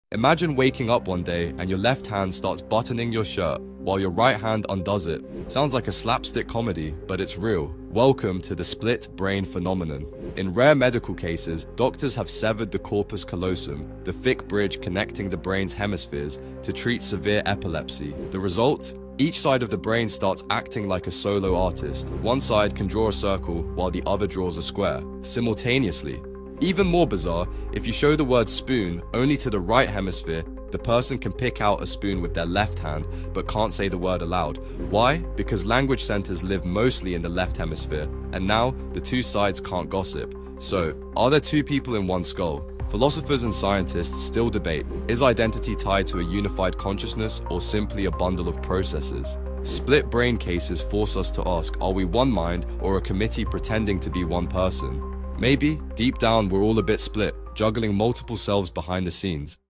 Split Brain: Two Minds, One Human?
Can one brain house two minds at once? The wild truth behind split brain cases will blow your mind!